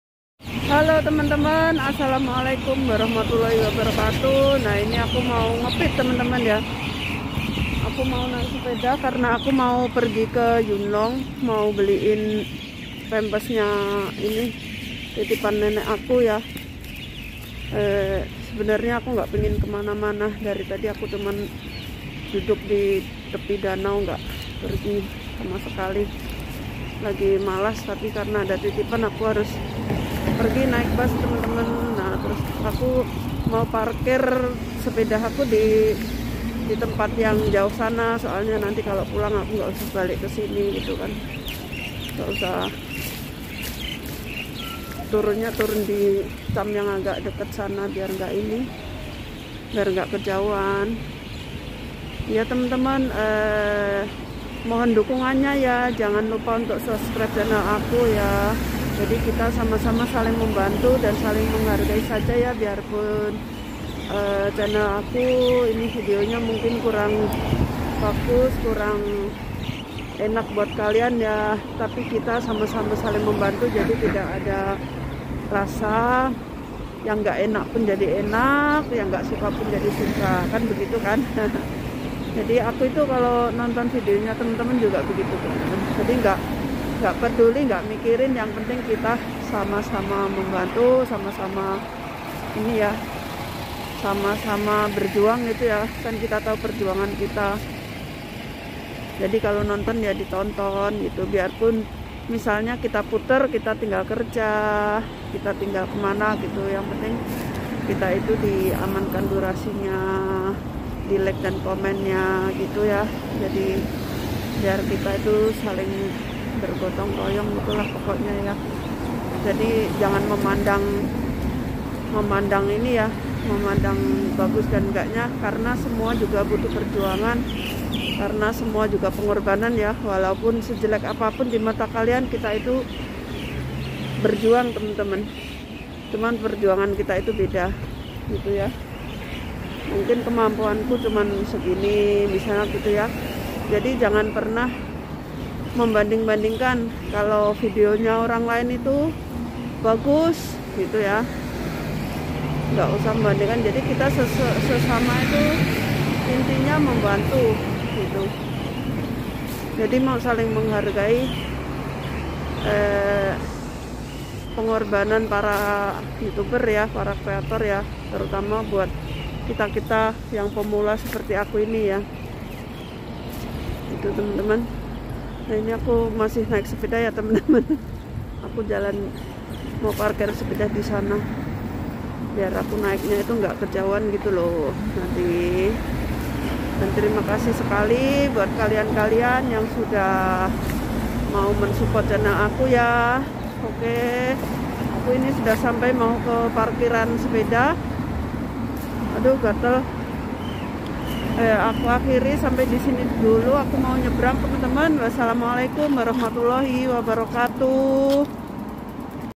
NAIK SEPEDA SAMBIL NGOCEH
ASSALAMUALAIKUM SELAMAT SORE TEMAN-TEMAN
SAYA BERCERITA KARENA SAYA MAU MAIN BASS UNTUK PERGI KE YUENLONG